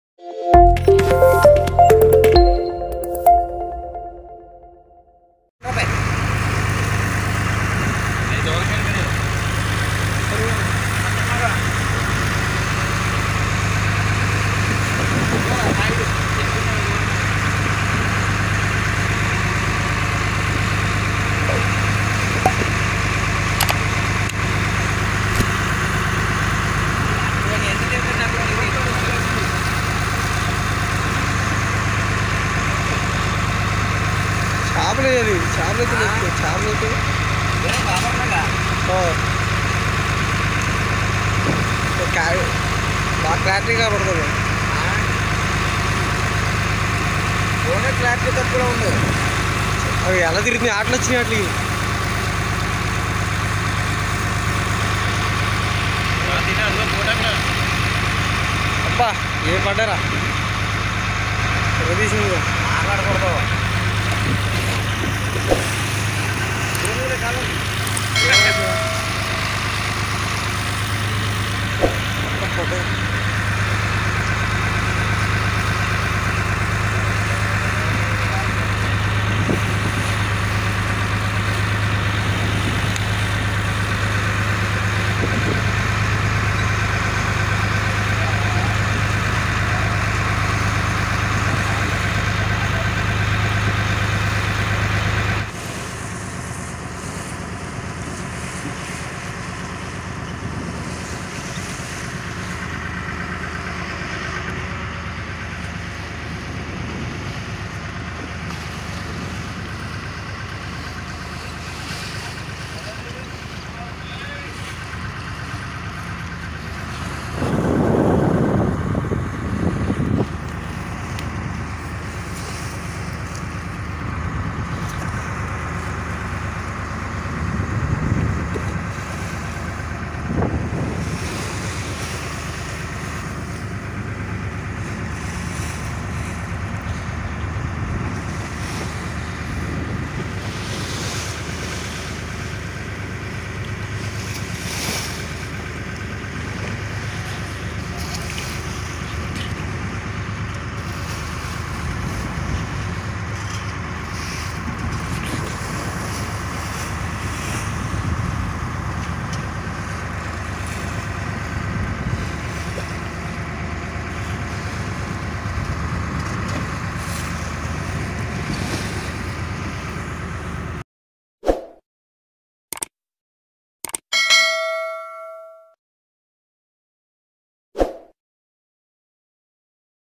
డాల్ఫిన్ల ఆటలు ll Amazing Wild Dolphins doing Spinner Jumpings ll Machilipatnam ll by ramstammu ll
డాల్ఫిన్ల ఆటలు ll dolphins games near Fishing boat ll Machilipatnam ll by ramstammu ll